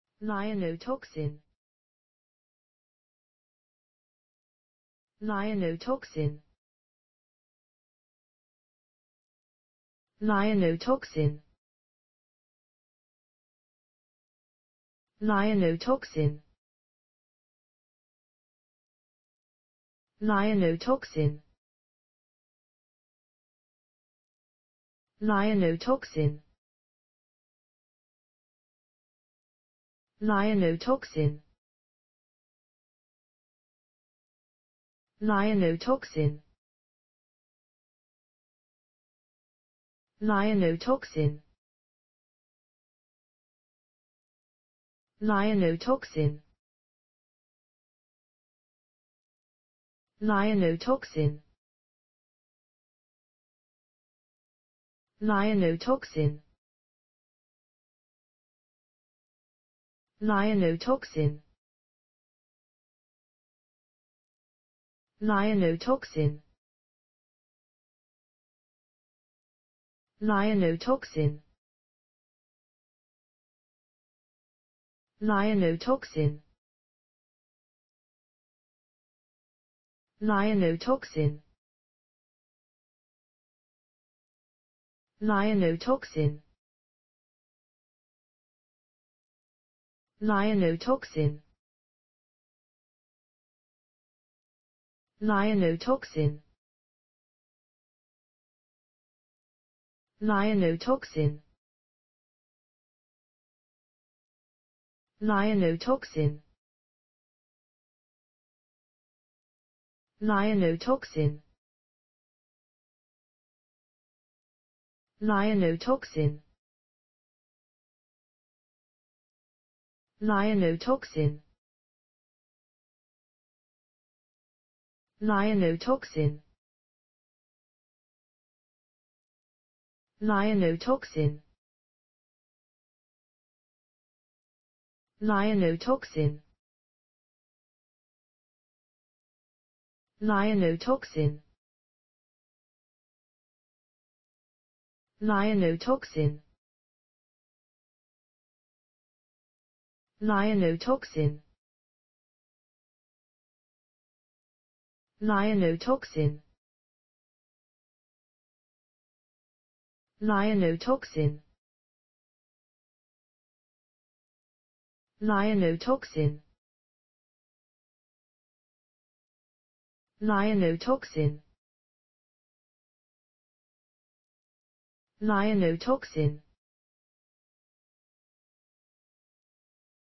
lienotoxin. How to pronounce the english word lienotoxin .Start with L. Learn from me.
Follow me!Learn from me how to pronounce the english word lienotoxin.This video shows you how to pronounce the English word lienotoxin .